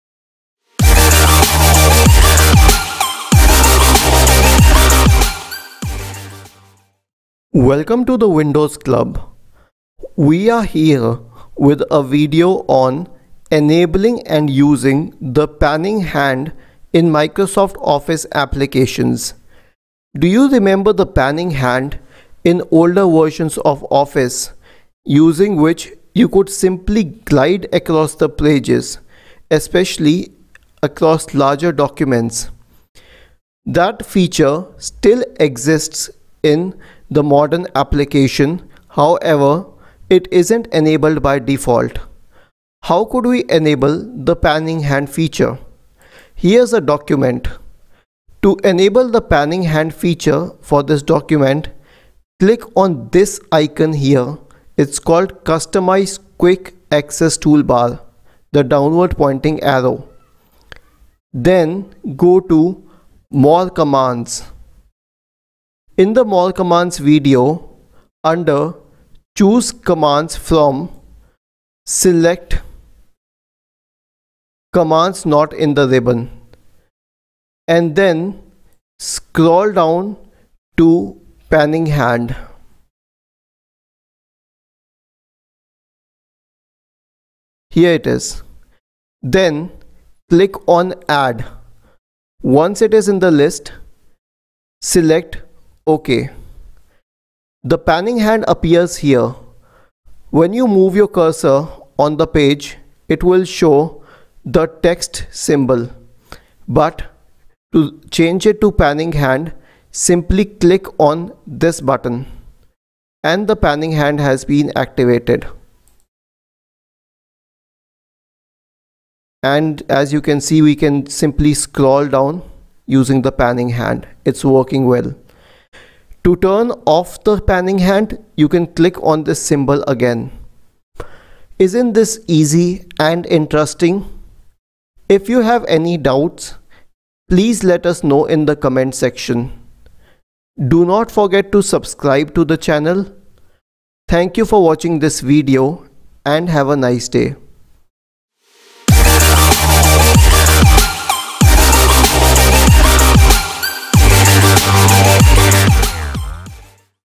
How to enable and use Panning Hand in Microsoft Office apps
4Generally, you can scroll down or up with your mouse wheel, or you can use the scroll bar on the right-hand side of your screen. However, if you want to use a different method to scroll up, down, right, and left in Word or other apps, Panning Hand can do the job for you.

By enabling this feature, you can see a hand-like icon on your screen. Later on, you can use the hand-icon as a mouse cursor, click on somewhere and move your mouse up or down to scroll the page accordingly.

To enable Panning Hand in Microsoft Office apps, follow these steps:

1] Open Word on your computer.

2] Click the Customize Quick Access Toolbar button.

3] Select the More Commands option.

4] Select Command Not in the Ribbon from the drop-down list.

5] Find out Panning Hand and select it.

6] Click the Add button.

7] Click the OK button to save.

8] Click on the new Panning Hand icon to enable and scroll.